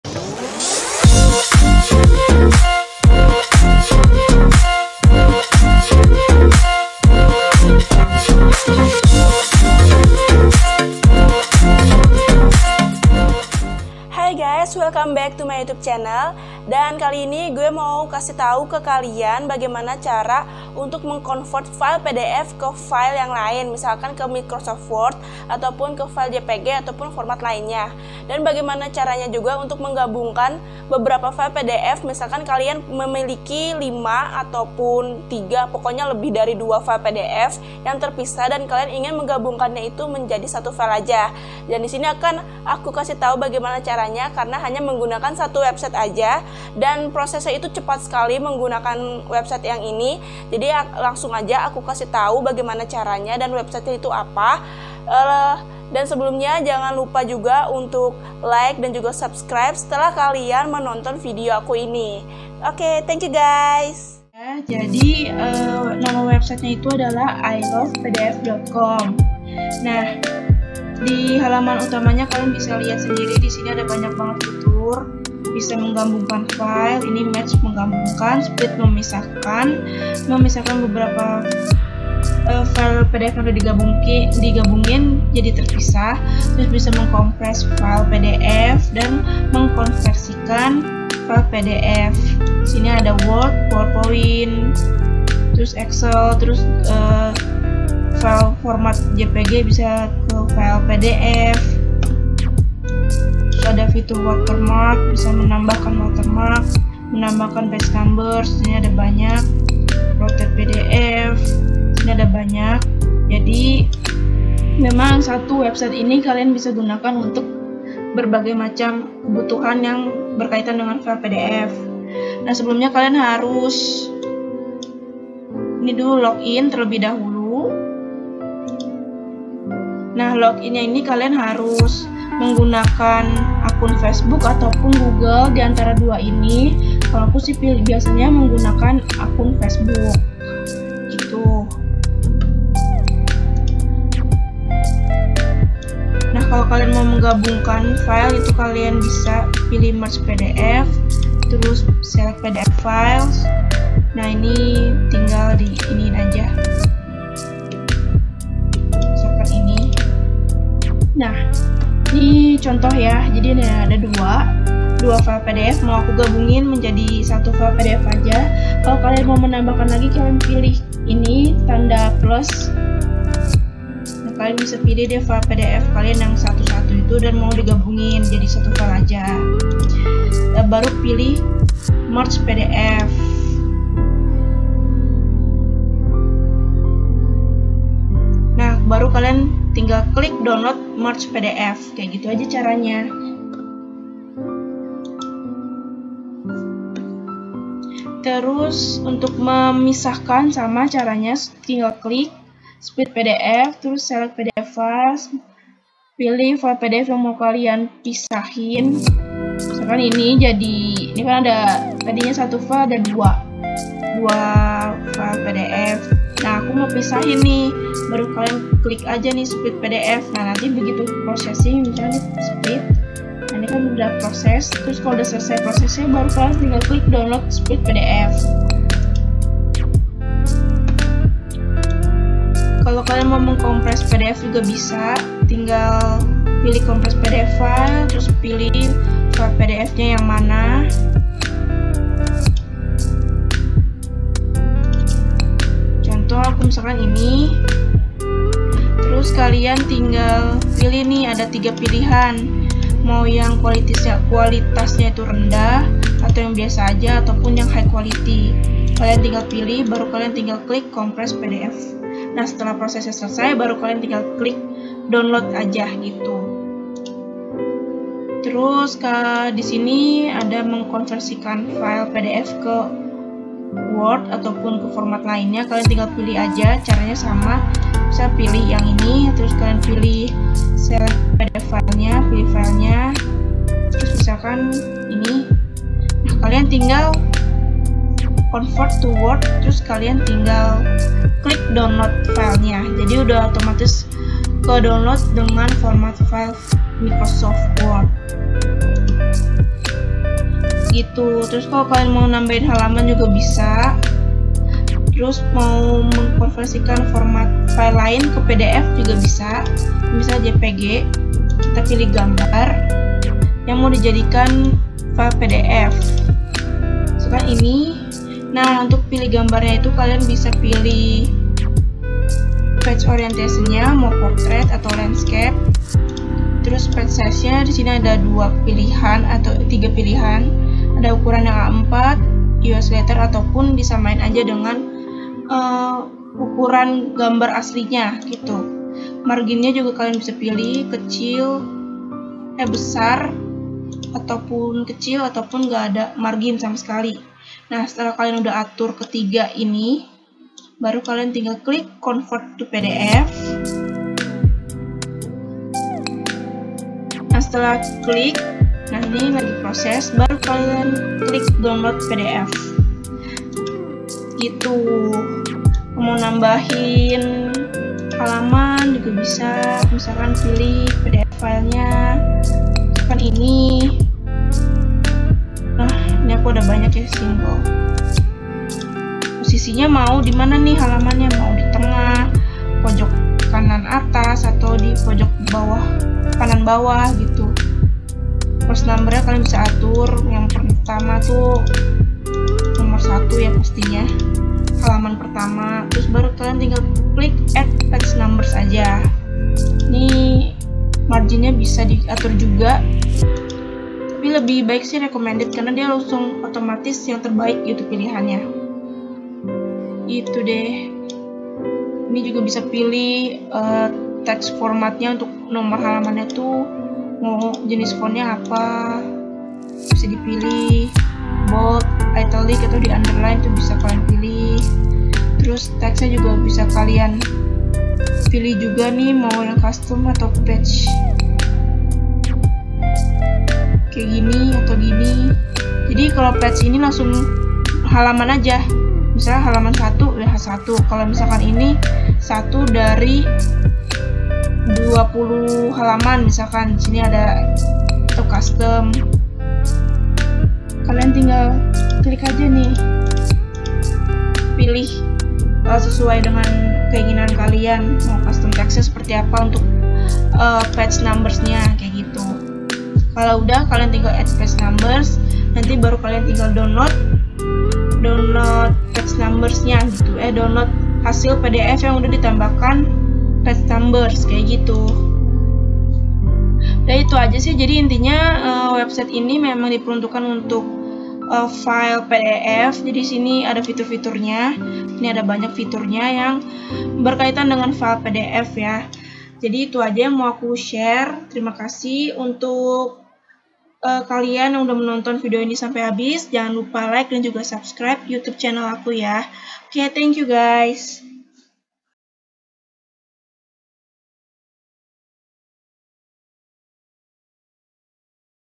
CARA CONVERT & MERGE FILE PDF DENGAN CEPAT
Video ini menjelaskan caea untuk convert dan merge. Mengkonversikan dan menggabungkan file PDF dengan cepat.